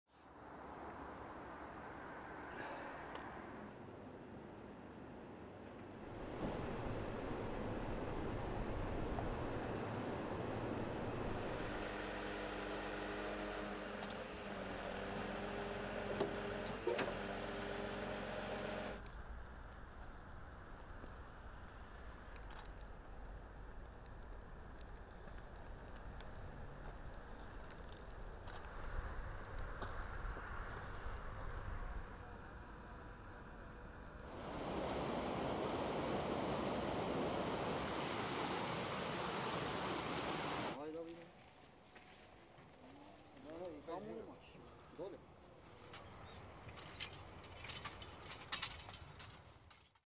Presas llenas de basura y ríos contaminados en los Balcanes
Las fuertes lluvias arrastran la basura río abajo en una zona cada vez más contaminada.